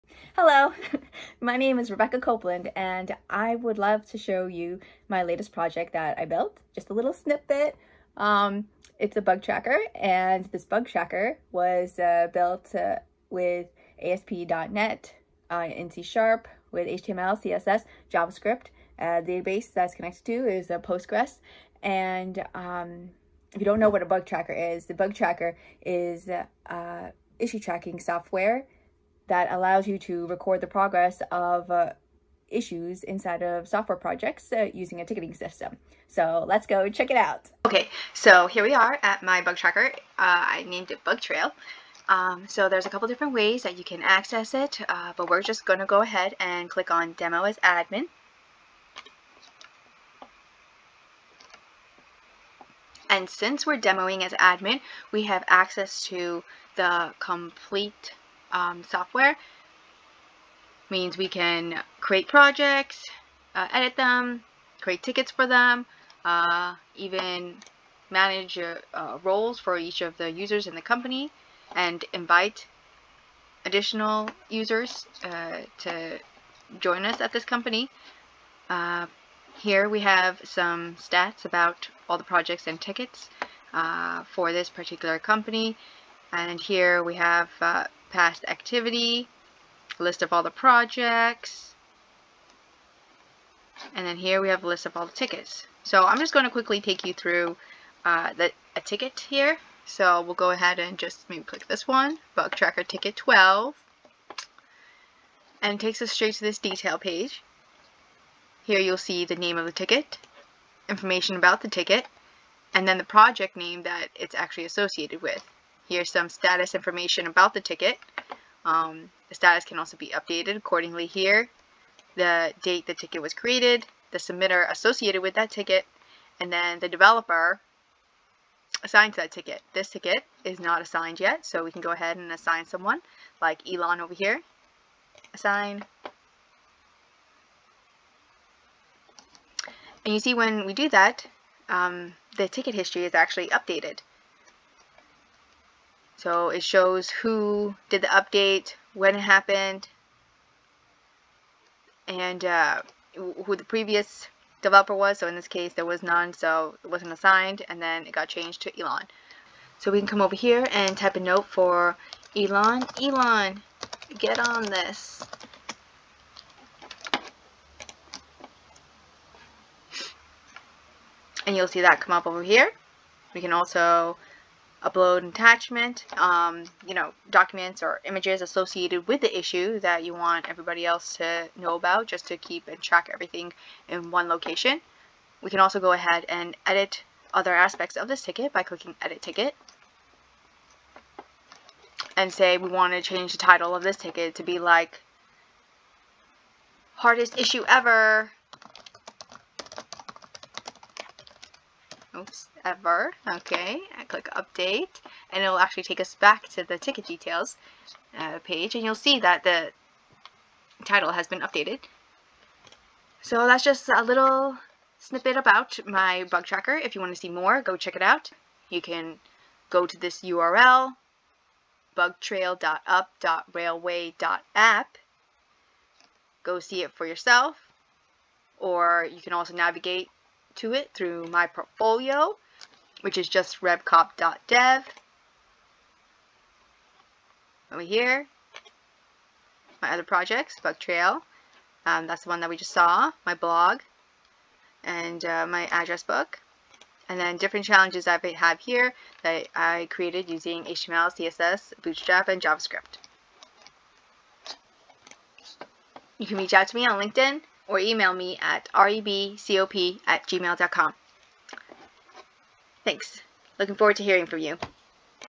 Bug Tracker Project Demo: Built With ASP.NET with C#
This is a small demo of my latest CRUD project, a bug tracker! It was built with ASP.NET Core in C#, HTML, CSS, JavaScript and connected to a Postgres database.

I'd love to hear from you, let me know what you think!